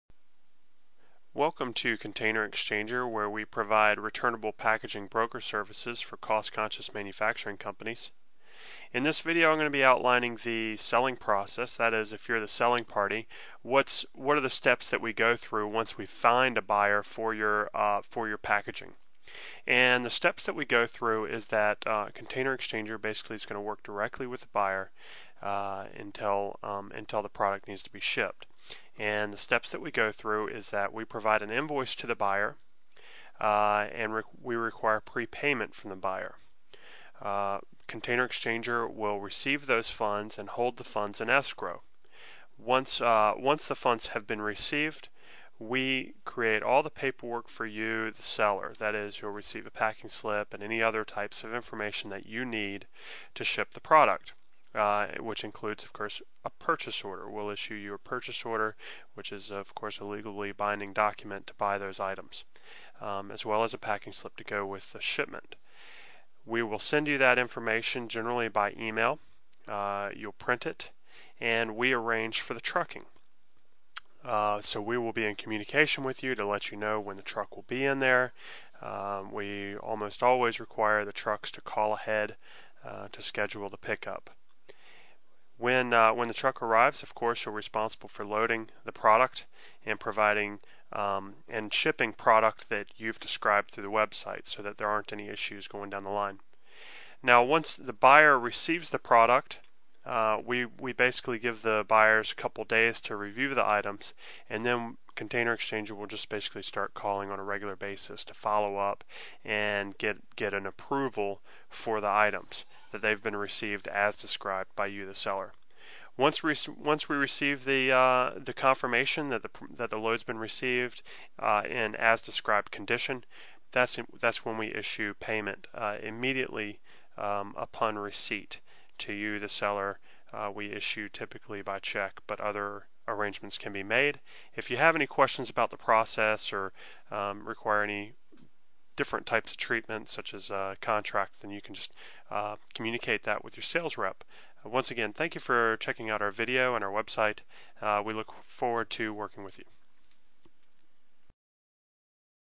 The Selling Process - Container Exchanger
This video outlines the papertrail and transaction process for used containers sold through